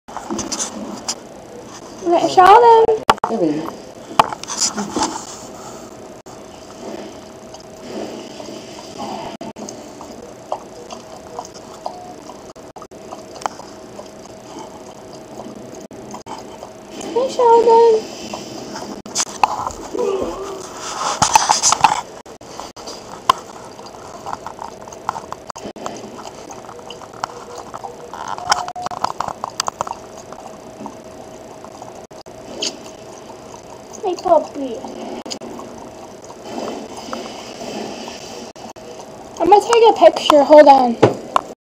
Sheldon the Sheltie in 2007 10 year old Sheltie
Sheldon (my Sheltie) drinking water.  My Mom and I are in the video, too.